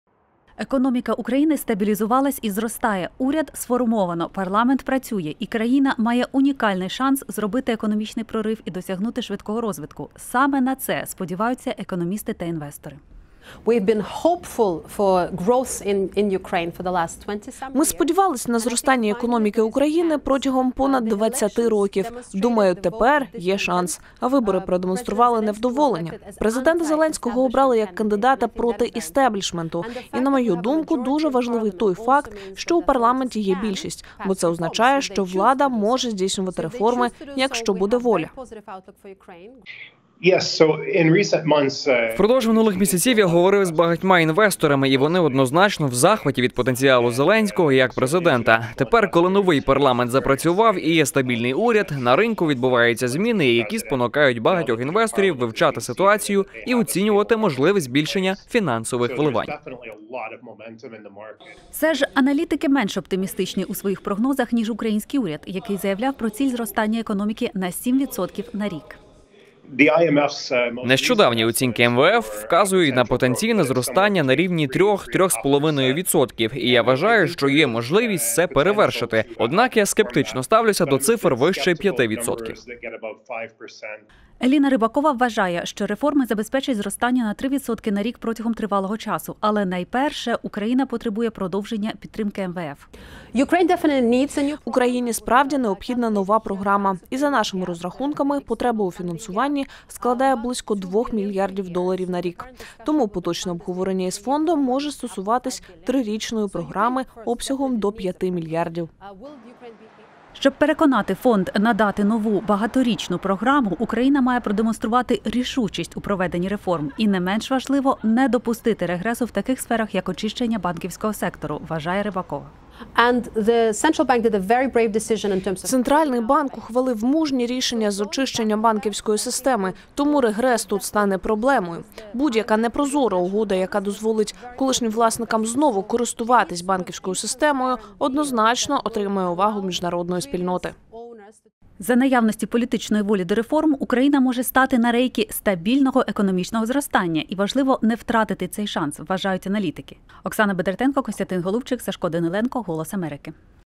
Україна має унікальний шанс зробити економічний прорив – американські економісти
Україна має унікальний шанс зробити економічний прорив і досягти швидкого розвитку – американські економісти.

За  роботою місії МВФ та реформаторськими ініціативами керівництва держави уважно спостерігають міжнародні інвестори.